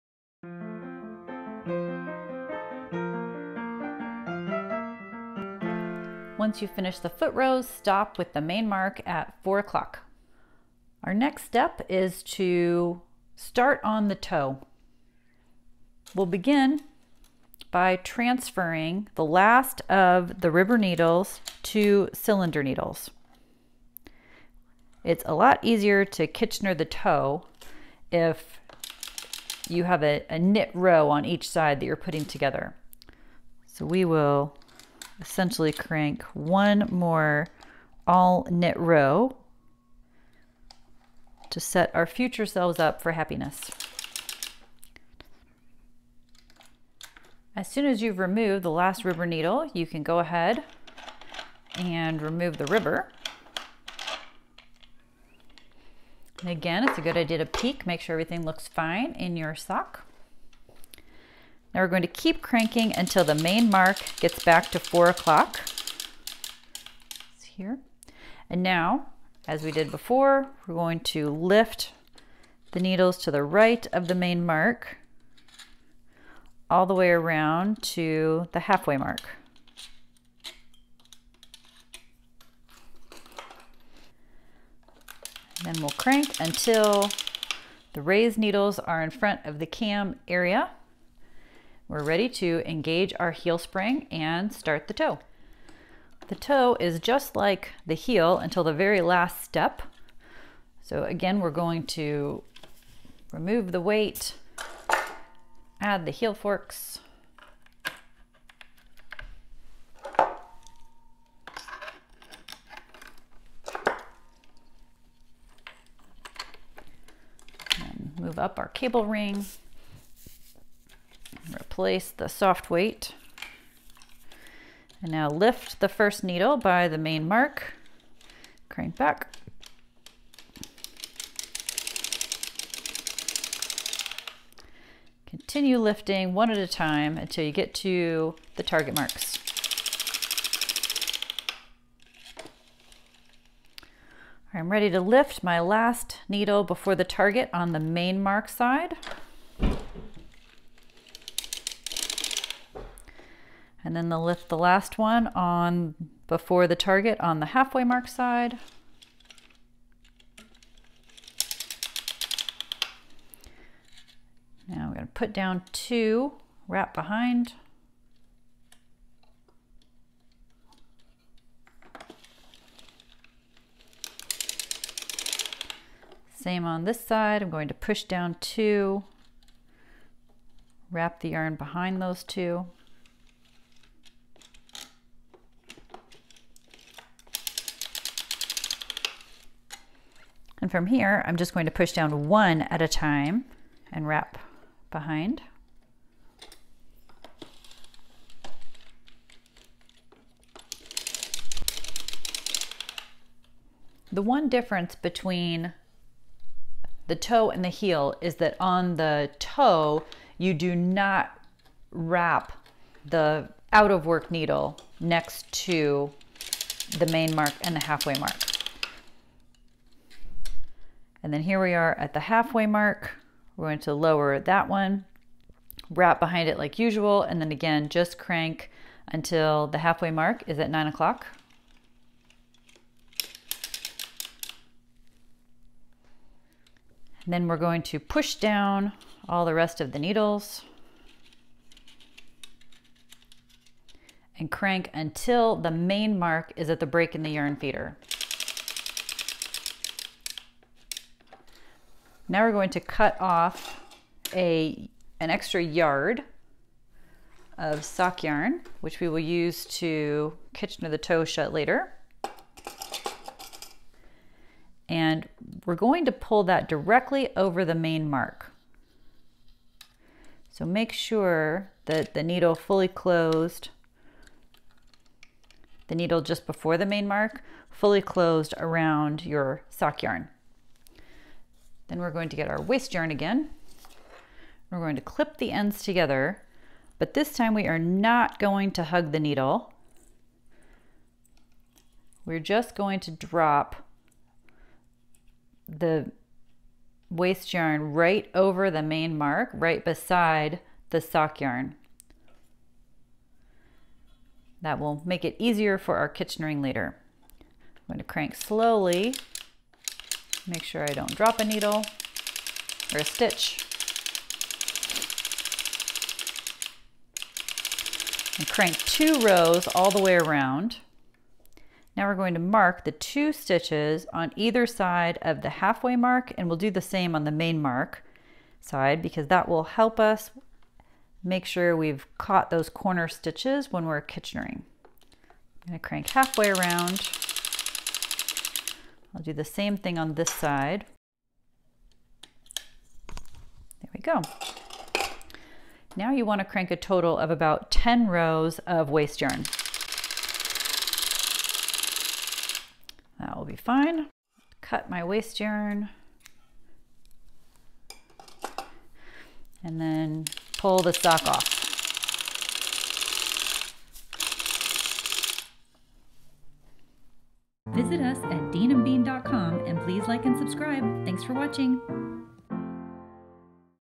Ribbed Sock Crank Along Part 8
This is part 8 of a crank along series to knit a ribbed sock on your Dean and Bean’s 2.0 Model Sock Machine and Ribber. In this video, after removing the ribber and getting set up for the toe, we’ll crank the toe, switch back to waste yarn, and remove the sock from the machine. The free ribbed sock pattern and full video series are available

We also provide videos on getting started with the ribber and troubleshooting common issues, which are available